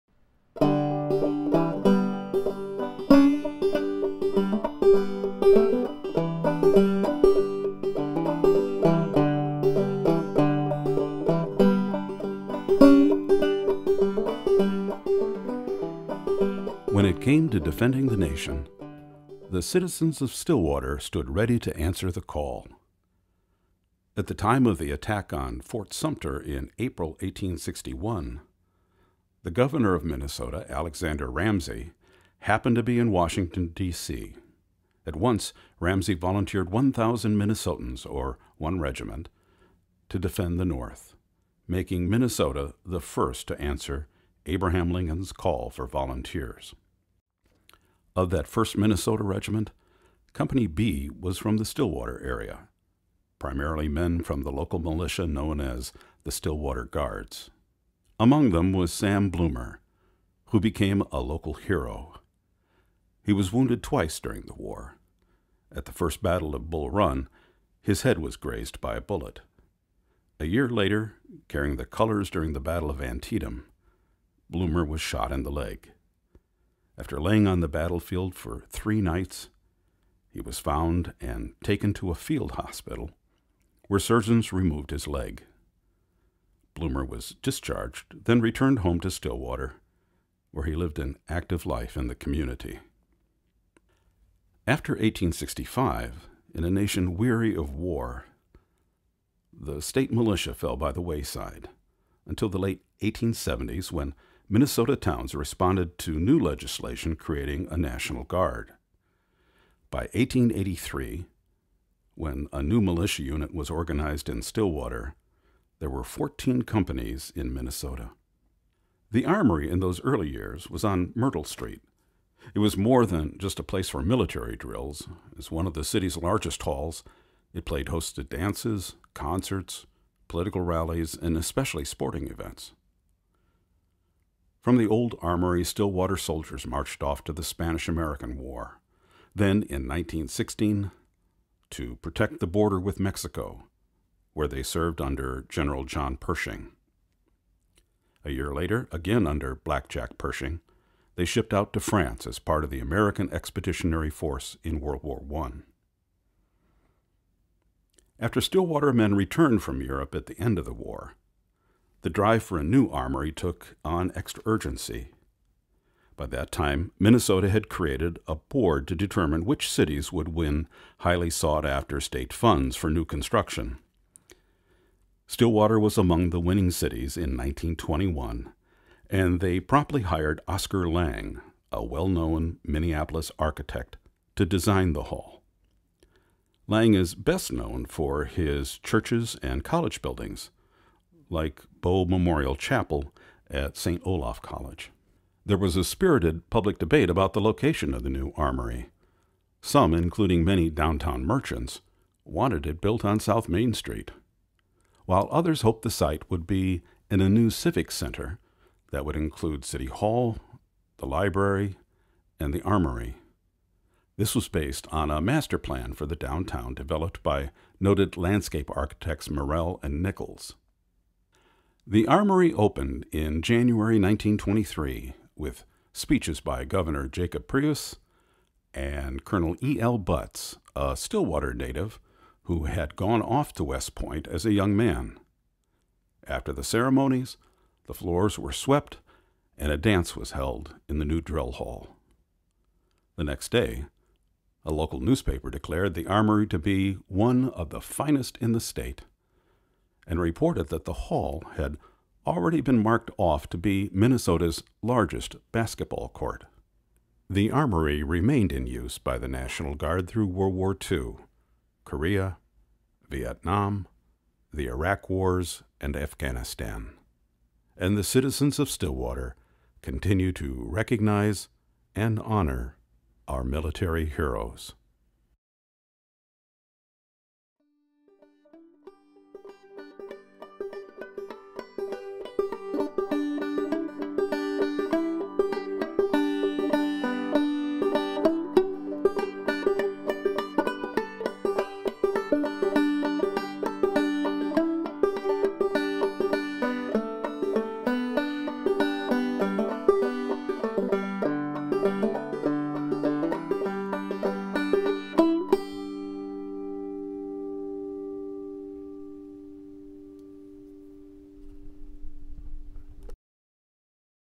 07 National Guard Armory (107 Chestnut Street)
The National Guard Armory was built in 1922. Stillwater has a long tradition with the National Guard, and after World War I, the city pressed for a new building. Oscar Lang, a well-known Minneapolis architect, designed the hall. It was used not just for military training, but also for public events, sports, and concerts.